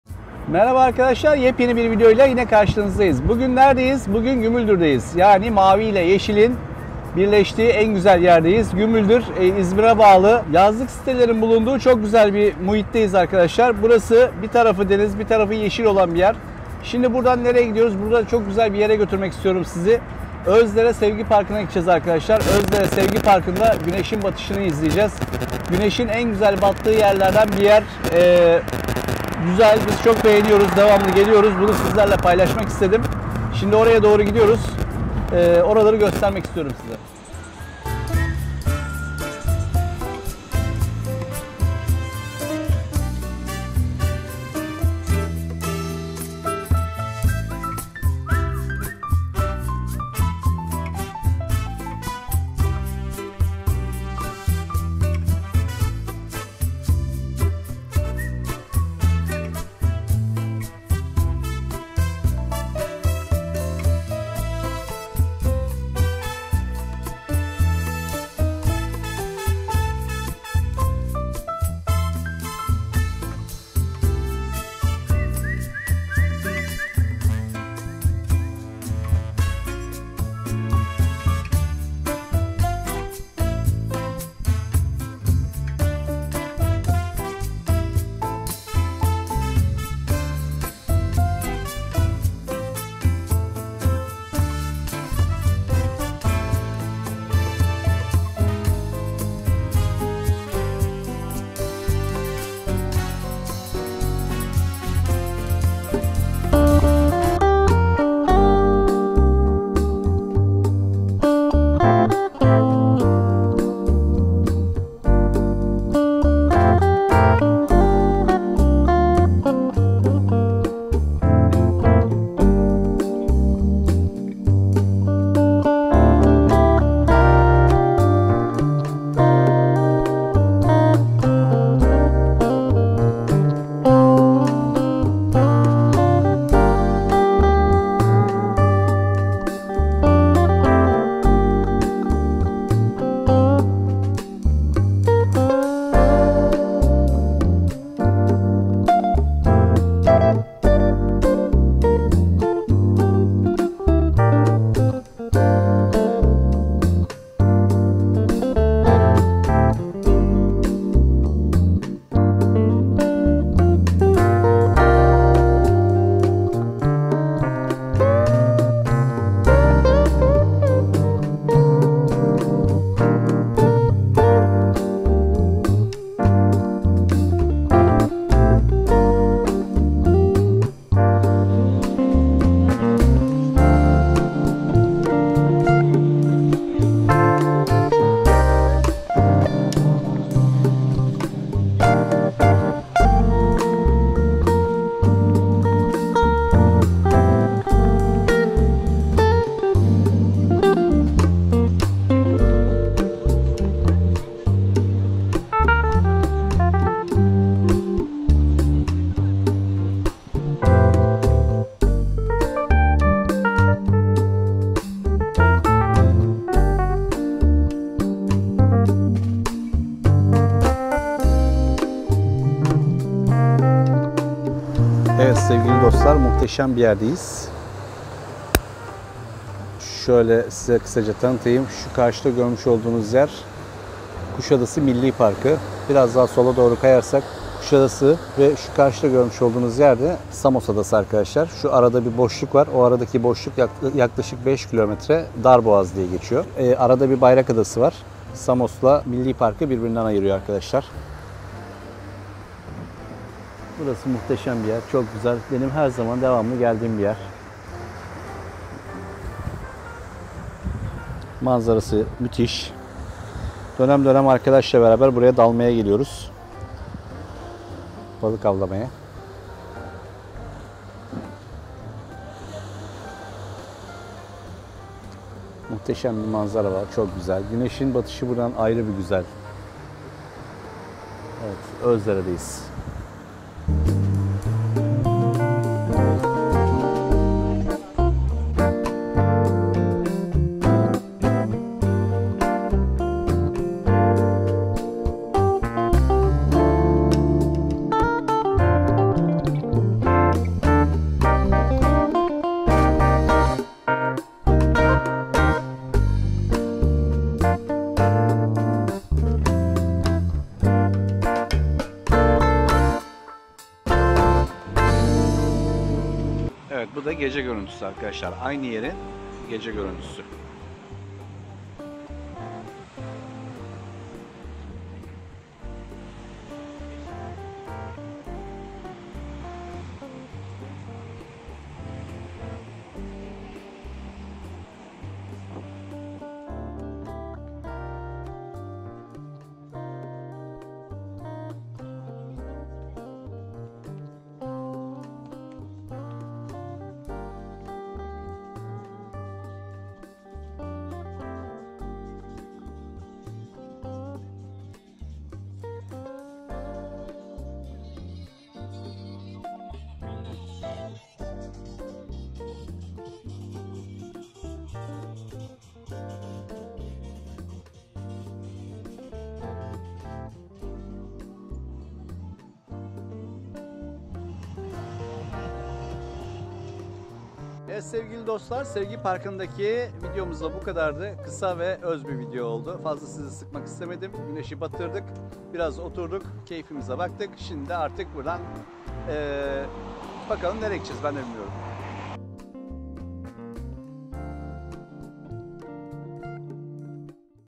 Özdere, İzmir Sevgi Parkı, Güneşi Batırıyoruz. Sunset. Turkey Holiday.
turkeyholiday, sunset, gümüldürözderesevgiparkı Ege nin En güzel Sahil lerinin Olduğu İzmir'in Sahil Kasabası Özdere, Ürkmez, Gümüldür ün Olduğu bölgede, Özdere den Ahmetbeyli ye giderkerken sağda tepe de Deniz kenarında bulunan sevgi parkı hem kafe cafe hemde restaurant olarak hizmet vermekte, ve Güneş batımının sunset en iyi izlendiği yerlerden birisidir. buyrun izleyin ve kendiniz görün.. Deniz, kum, güneş, Özdere Ürkmez Gümüldür..
Olumlu Yada olumsuz yorumlarınızı Sormak istediğiniz Soruları Yorumlar kısmında yazabilirsiniz...